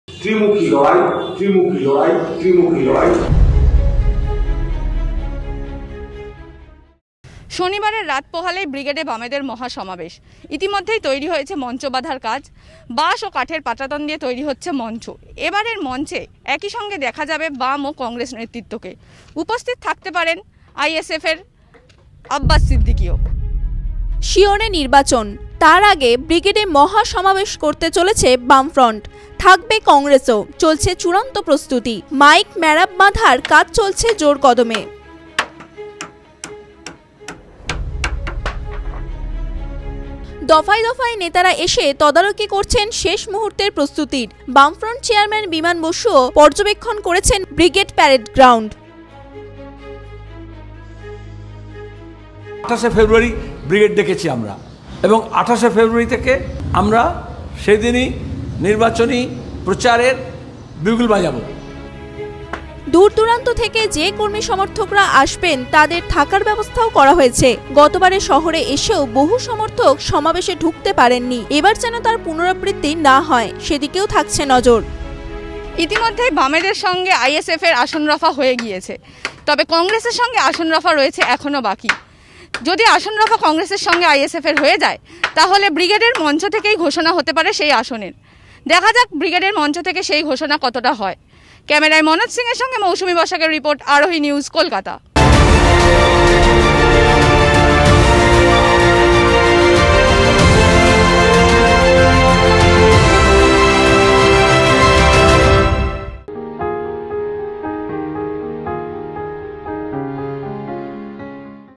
আঠাশের ব্রিগেডের আগে ব্যস্ততা তুঙ্গে
Brigade Meeting on 28th february called by Left, Cong | বিধানসভা নির্বাচনের আগে ২৮ ফেব্রুয়ারি বামেদের ব্রিগেড। দলীয় নেতা-কর্মী থেকে সমর্থকদের উজ্জীবিত করতে ব্রিগেড সমাবেশকেই পাখির চোখ করছে বামেরা. জোরকদমে চলছে তারই প্রস্তুতি.

@Aarohi News